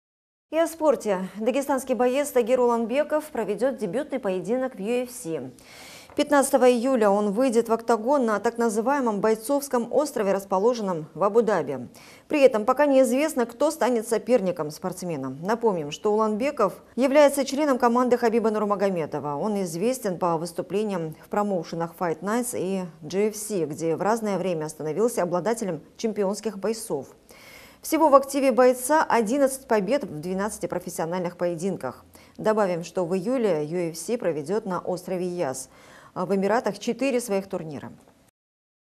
Тагир Уланбеков дебютирует в UFC
Дагестанский боец Тагир Уланбеков проведет дебютный поединок в UFC. 15 июля он выйдет в октагон на, так называемом, «Бойцовском острове», расположенном в Абу-Даби.
При этом пока неизвестно, кто станет соперником спортсмена.

Напомним, что Уланбеков является членом команды Хабиба Нурмагомедова. Он известен по выступлениям в промоушенах Fight Nights и GFC, где в разное время становился обладателем чемпионских поясов. Всего в активе бойца 11 побед в 12-ти профессиональных поединках.

Добавим, что в июле UFC проведет на острове Яс в Эмиратах четыре своих турнира.